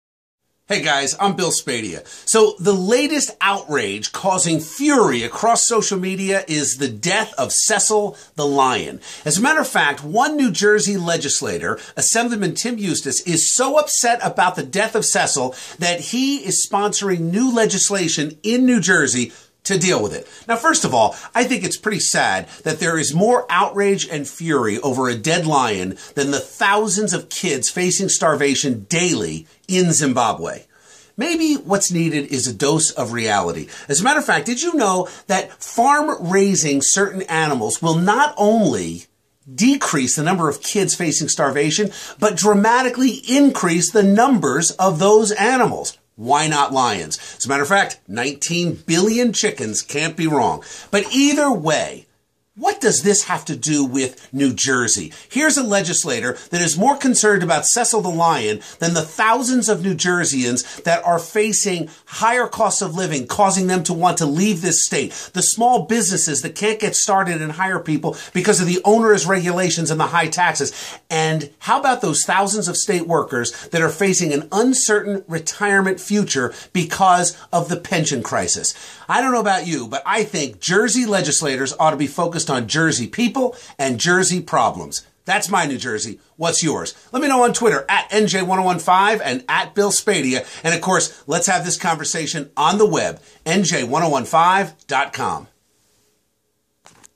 Cecil the Lion's Death - NJ has other things to be concerned with
Bill Spadea talks about the outrage surrounding the death of Cecil the Lion and how New Jersey should be more concerned with other issues right now.